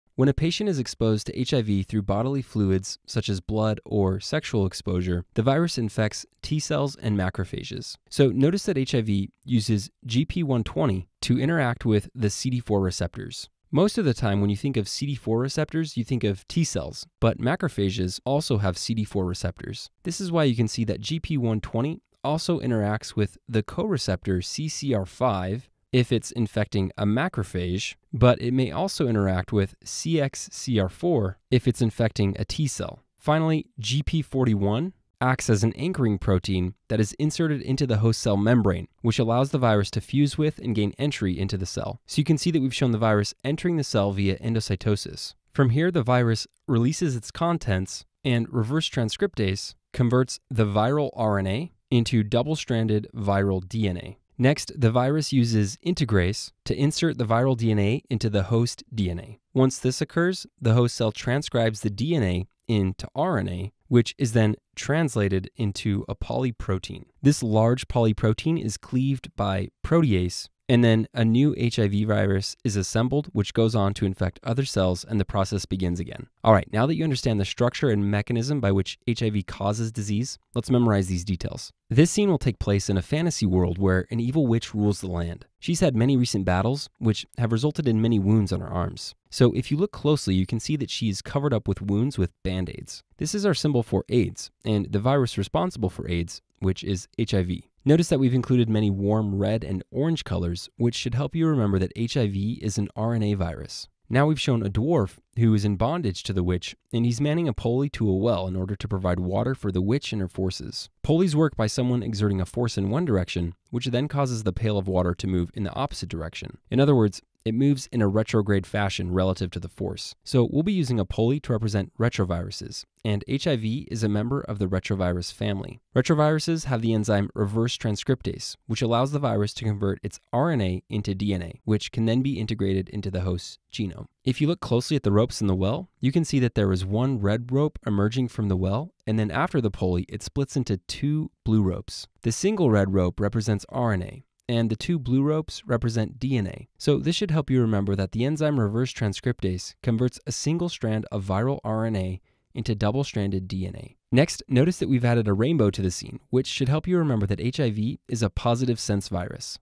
Human Immunodeficiency Virus — The master of immune evasion
For USMLE for beginners, HIV illustrates viral latency, immune escape, and AIDS-defining conditions. Understanding these mechanisms makes other viral topics clearer and Step 1 questions easier to tackle.